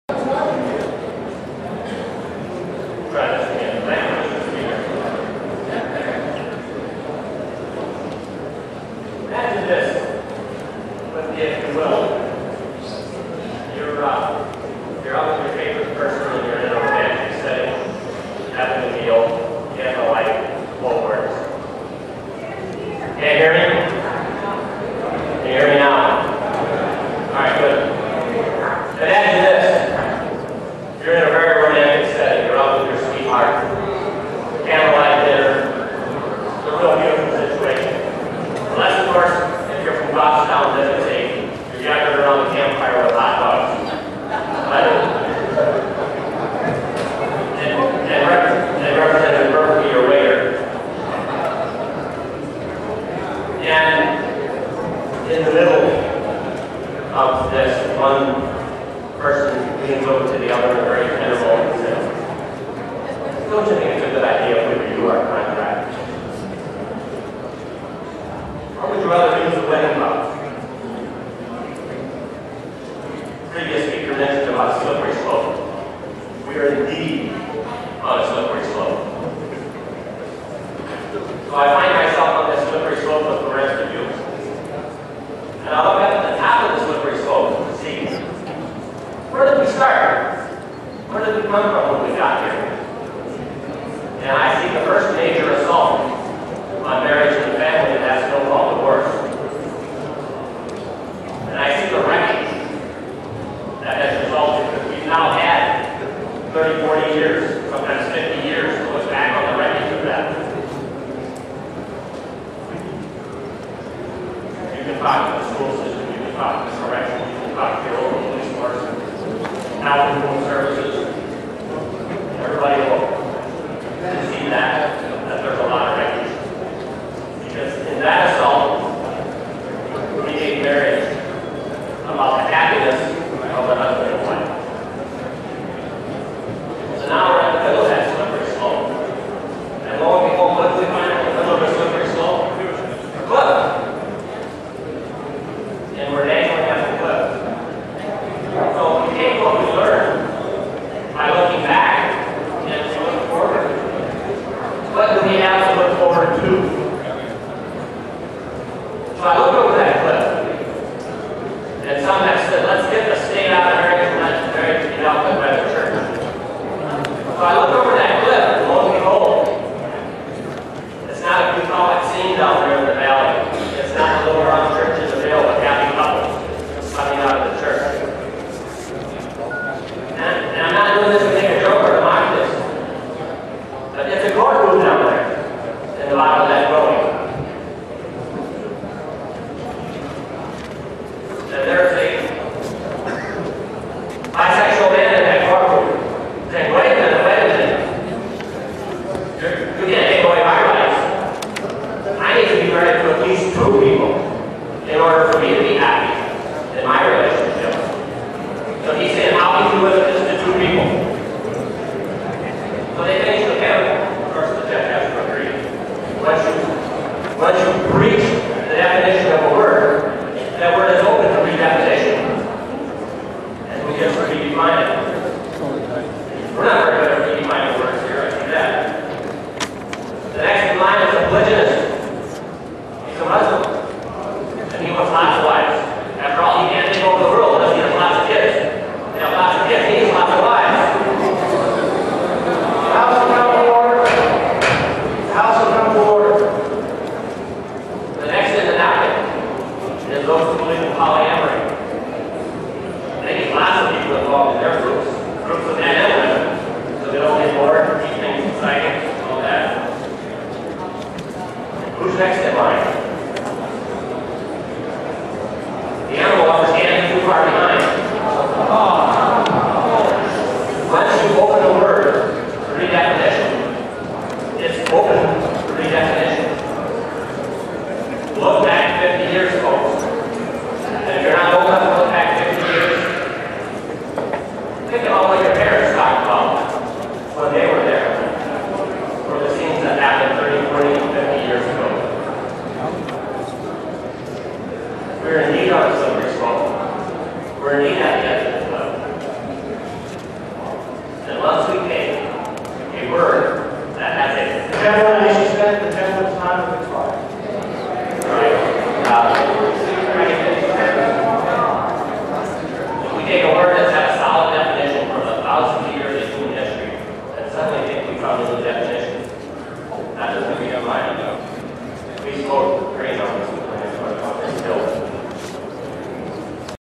NH Rep. Warren Groen (R-Rochester) on marriage repeal.
Rep. Groen makes the slippery slope argument against marriage equality, citing bigamy, polyamory, Islam and zoophilia until he is gaveled out of time, thus depriving the world of untold profound insights he is unable to unburden himself of.

March 21, 2012, NH House floor debate on HB437, about 4pm. Worse quality official House video is available